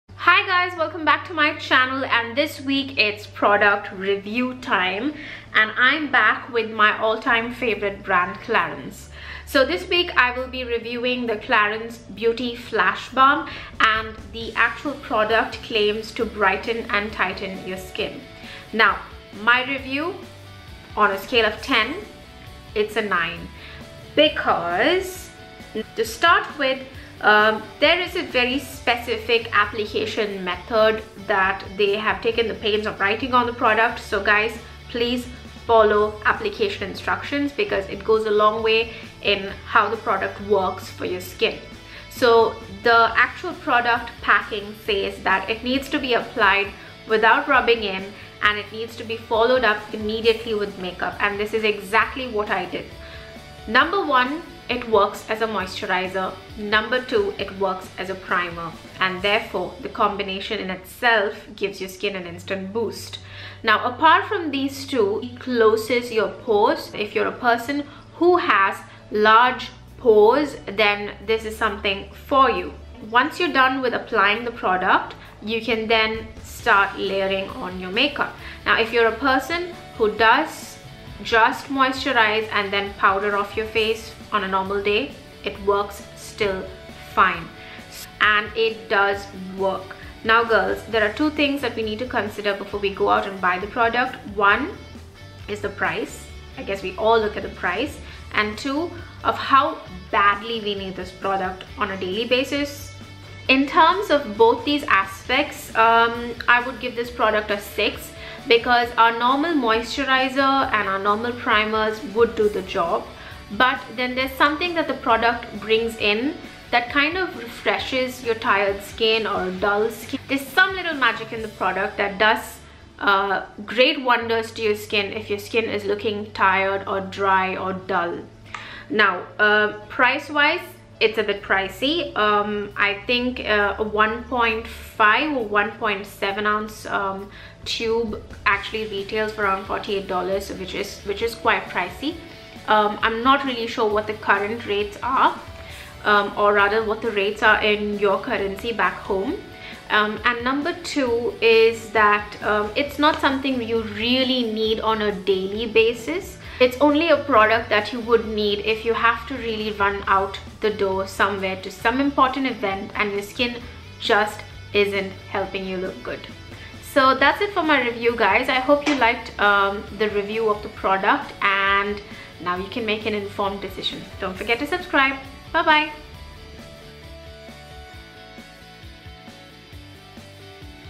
🎙️ SAY WHAT? | CLARINS BEAUTY FLASH BALM | BRIGHTENS AND TIGHTENS | REVIEW | INDIAN MEDIUM SKINTONE
Review;
a) High price point $$$
b) Specific application method (without rubbing in and follow up immediately with makeup)
c) Tube packing
d) Works as moisturizer and primer
e) Closes pores
f)  Refreshes and brightens tired dull skin

Common Questions;
What does it do?
Is it used as a mask?
Brightens and tightens?